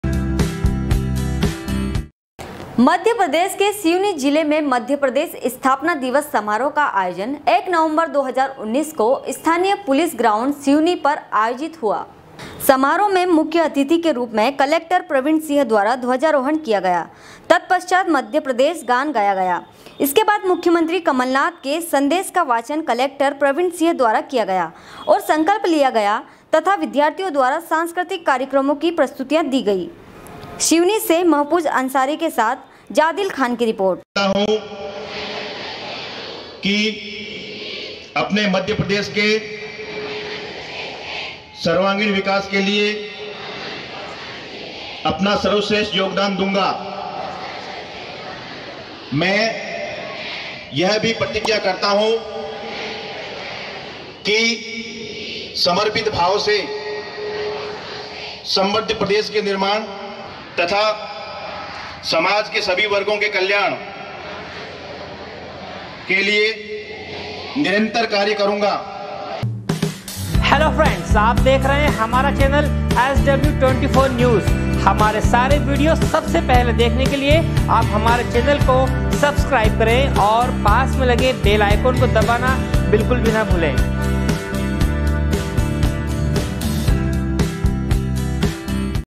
sw24news sivni, 01.11.2019
मध्यप्रदेष के सिवनी जिले में म.प्र. स्थापना दिवस समारोह का आयोजन 1 नवंबर 2019 को स्थानीय पुलिस ग्राउण्ड सिवनी पर आयोजित हुआ सिवनी से महफूज अंसारी कि साथ जादिल खान की रिपोर्ट#sw24news